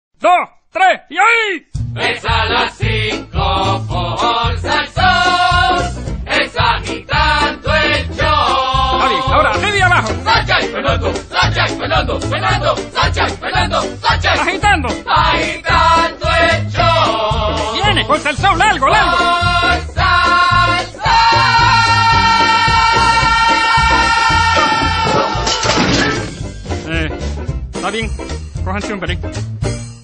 Agitando el Show (animado)
Comercial animado para show de radio "Agitando el Show". De lunes a viernes por SalSoul.